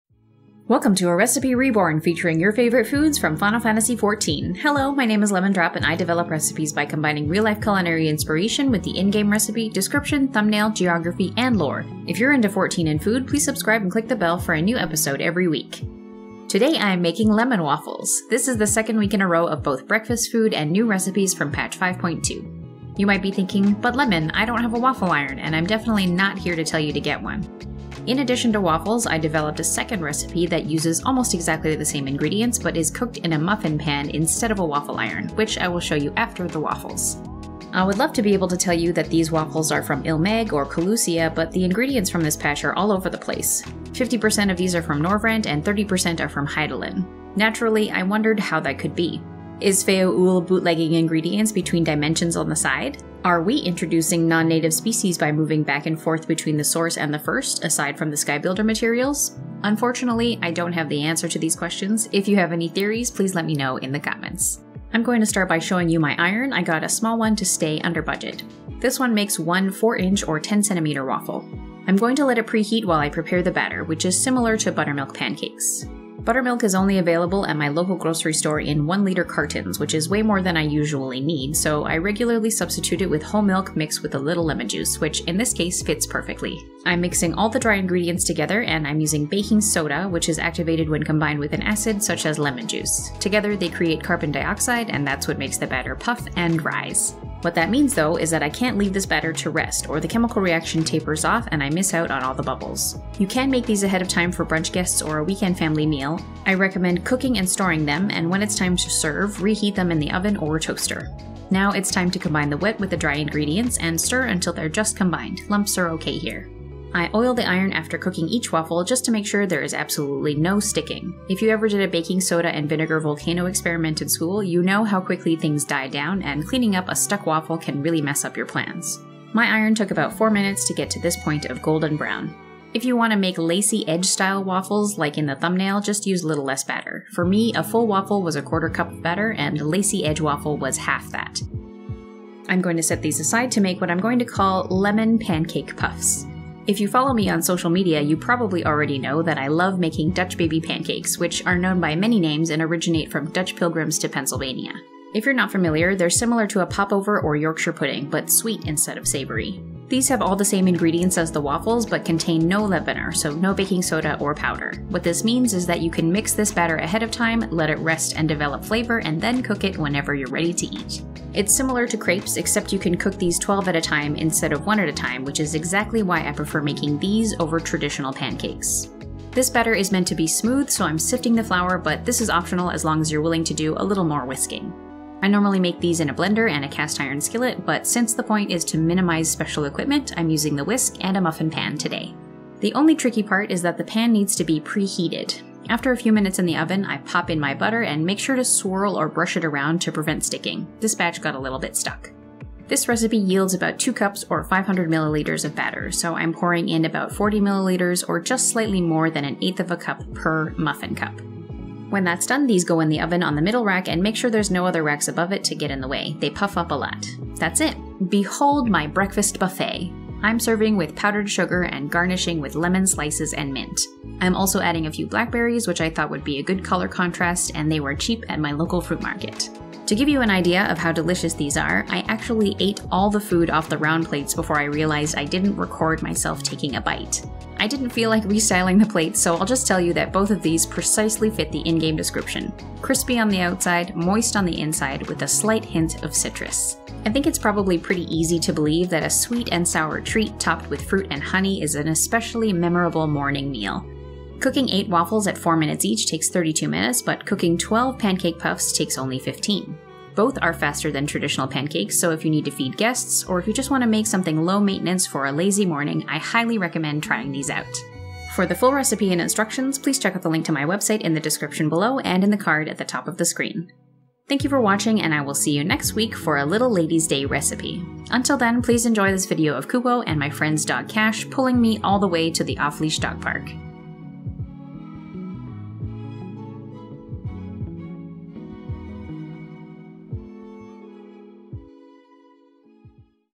Lemon Waffle | Cooking Final Fantasy XIV Food
Learn how to make FFXIV style waffles with a hint of lemon. For those of you who don't have a waffle iron, I have a special treat for you too! Recipe